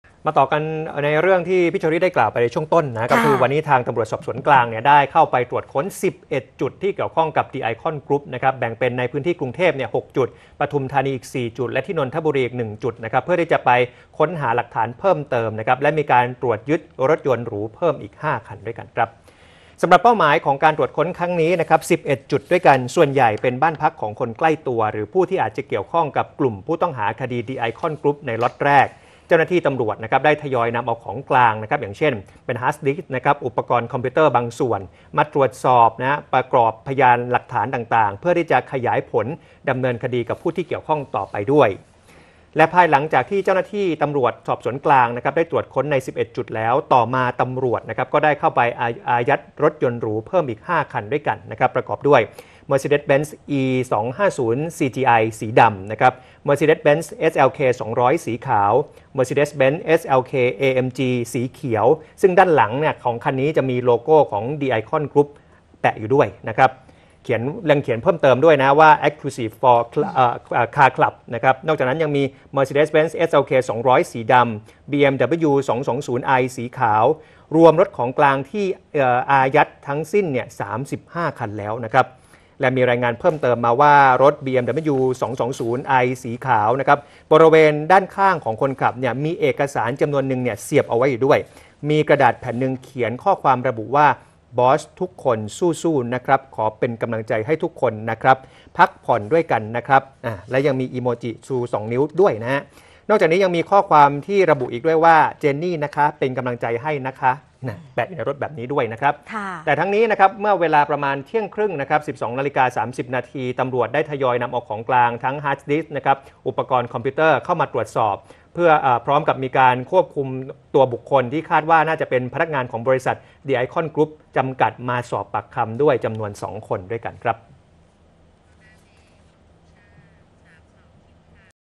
ตำรวจสอบสวนกลาง ค้น 11 จุด หาหลักฐานคดี THE ICON ข่าวค่ำ วันที่ 22 ตุลาคม 2567
ขณะเดียวกัน วันนี้ ตำรวจสอบสวนกลาง ได้เข้าตรวจค้น 11 จุด ที่เกี่ยวข้องกับคดี "ดิไอคอน กรุ๊ป" แบ่งเป็นพื้นที่ กทม. 6 จุด ปทุมธานี 4 จุด และนนทบุรี อีก 1 จุด เพื่อค้นหาหลักฐานเพิ่มเติม พร้อมตรวจยึดรถยนต์หรูเพิ่มอีก 5 คัน